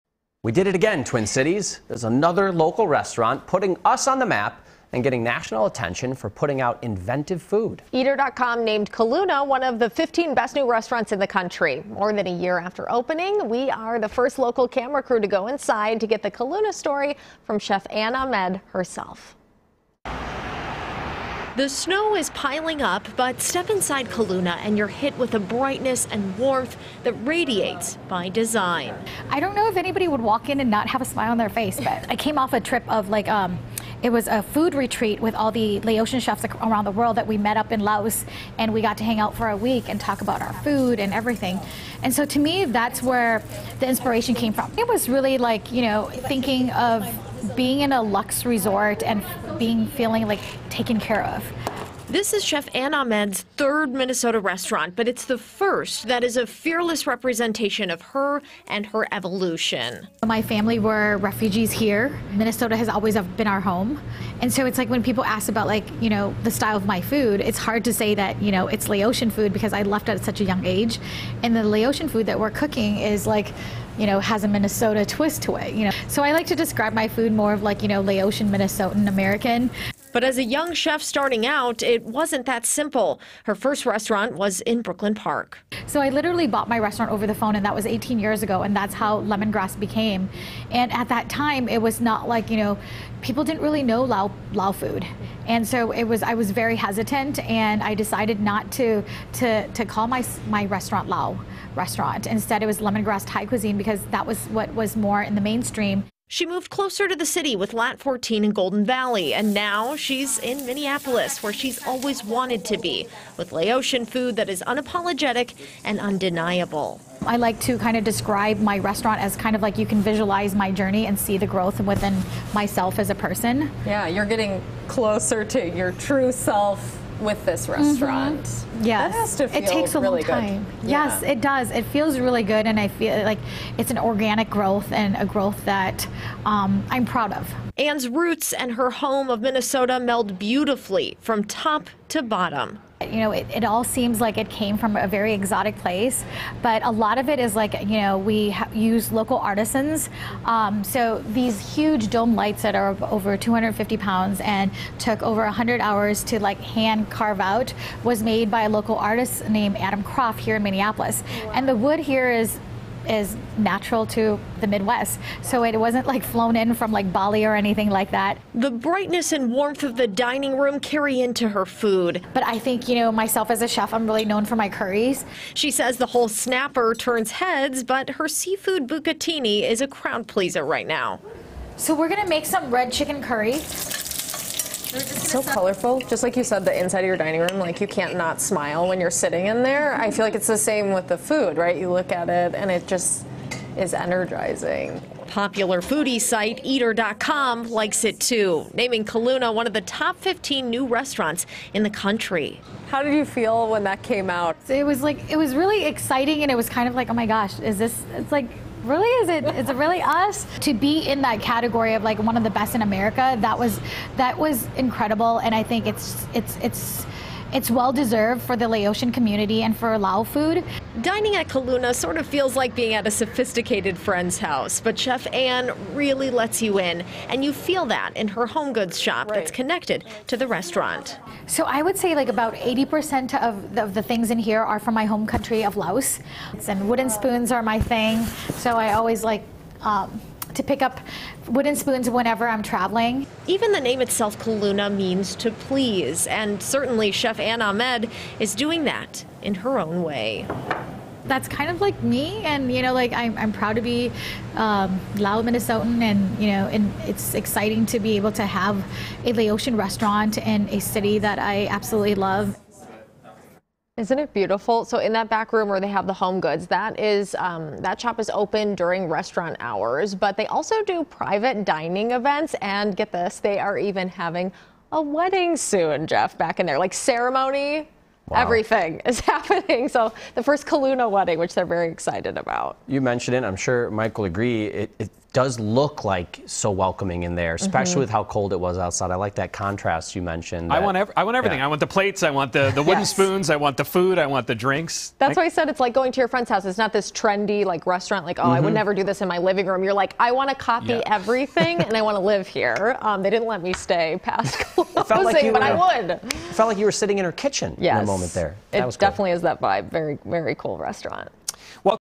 Khaluna named one of the 15 best new restaurants in U.S.
There’s another local restaurant putting Minneapolis on the map.